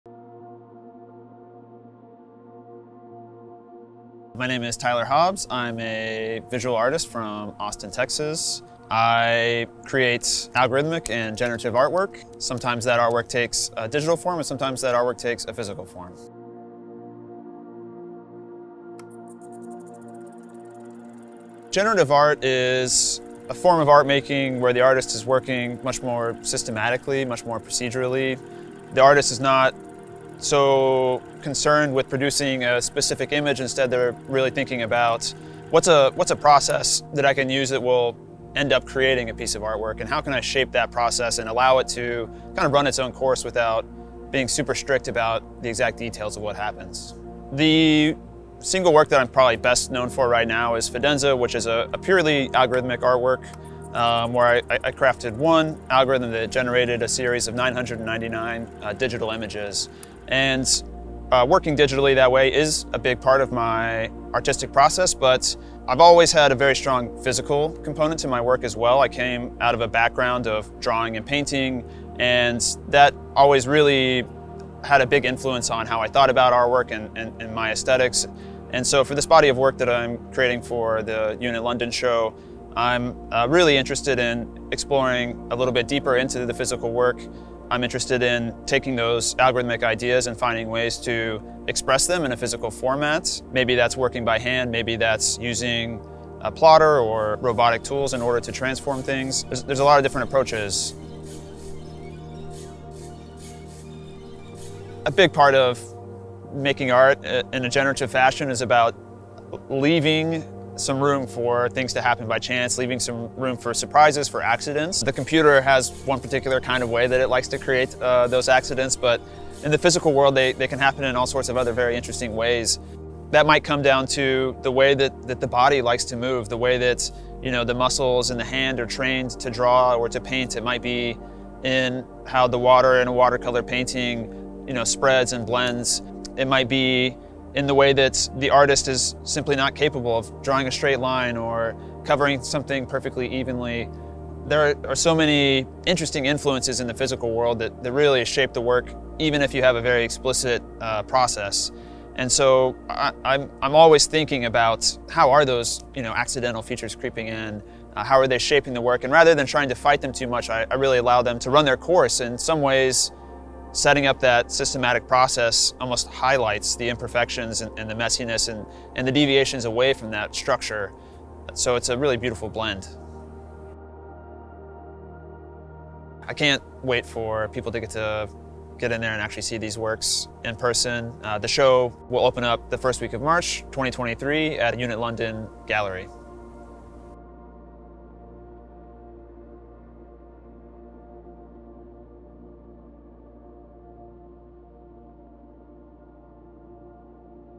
Introducing American Generative Artist Tyler Hobbs
Tyler Hobbs (b.1987) is a visual artist from Austin, Texas. His work focuses on computational aesthetics, how they are shaped by the biases of modern computer hardware and software, and how they relate to and interact with the natural world around us. By taking a generative approach to art making, his work also explores the possibilities of creation at scale and the powers of emergence.

Hobbs’ most notable project, Fidenza, is a series of 999 algorithmically generated works comprising one of the most sought-after fine art NFT collections of all time. His drawings, paintings, and digital works have been privately collected around the world. In 2021, Hobbs was part of the group exhibition, titled Transformations, at Unit London. He has presented two solo exhibitions, Progress (2018) at Galería Dos Topos in León, Mexico, and Incomplete Control (2021) at Bright Moments Gallery in Manhattan, New York. Additionally, his work was included in a Christie’s auction in 2021, and a Sotheby’s auction in 2022. Hobbs holds a BS in Computer Science from the University of Texas at Austin.

'Mechanical Hand' by Tyler Hobbs is coming soon to Unit London.

Videography by The Visual Brain